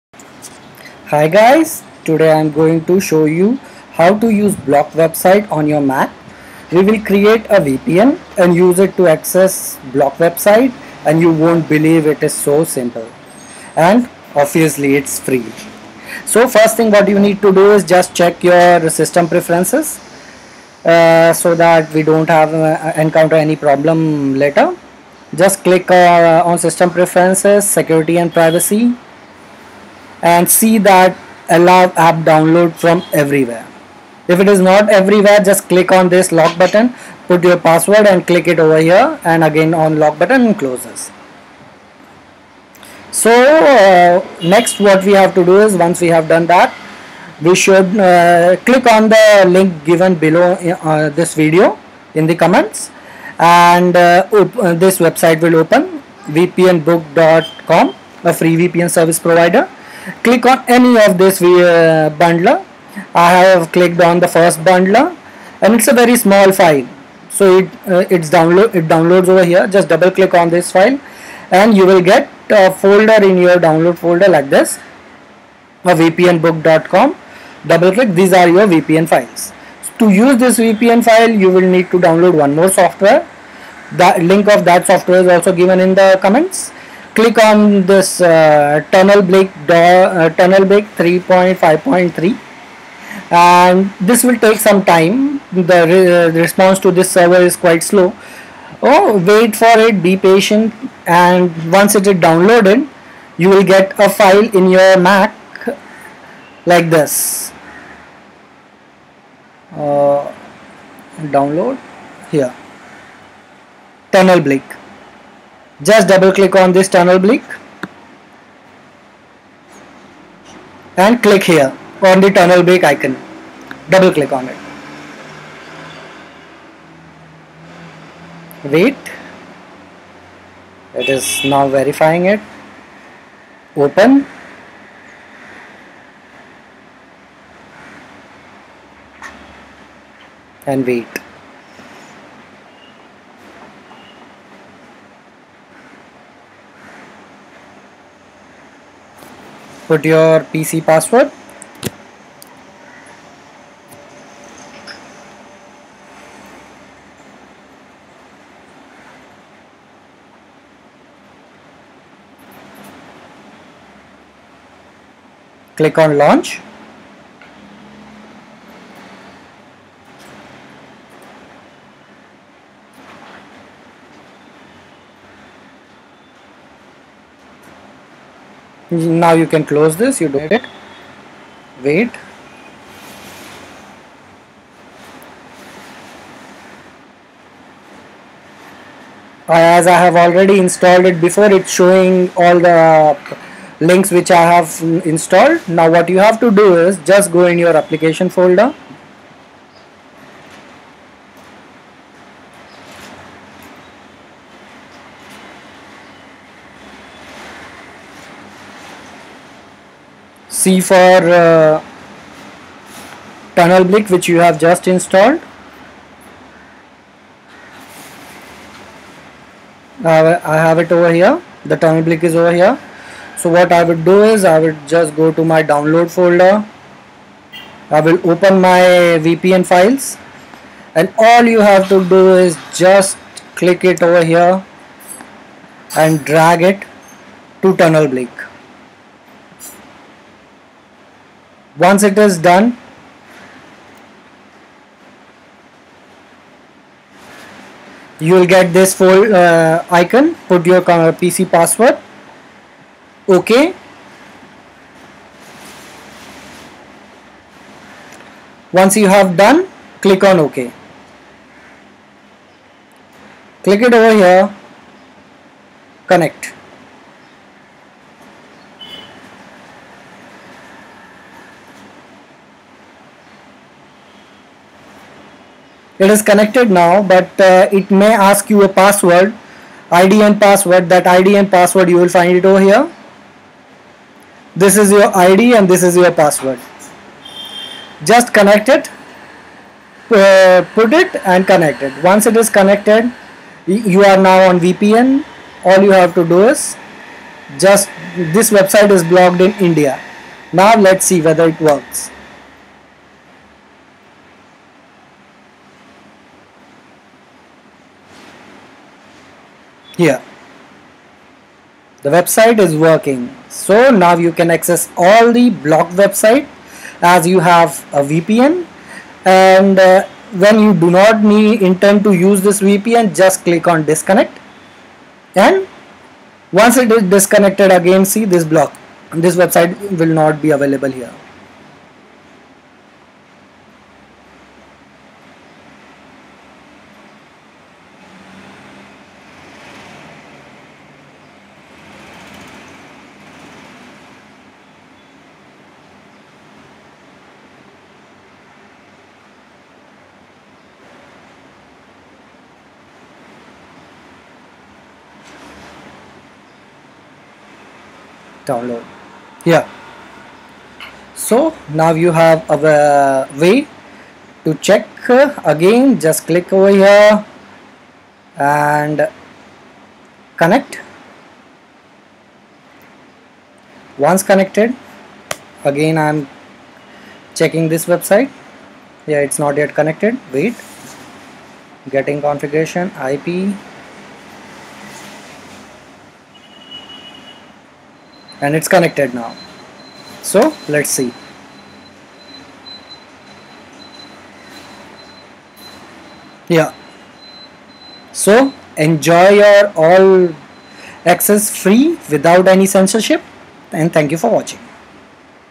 VIEW BLOCKED WEBSITE ON MAC, INSTALL FREE VPN ON MAC, SET VIRTUAL PRIVATE NETWORK (VPN) ON MAC
Hi guys everyone wants to use internet without censorship. this video will help to access all the block content on internet. use wisely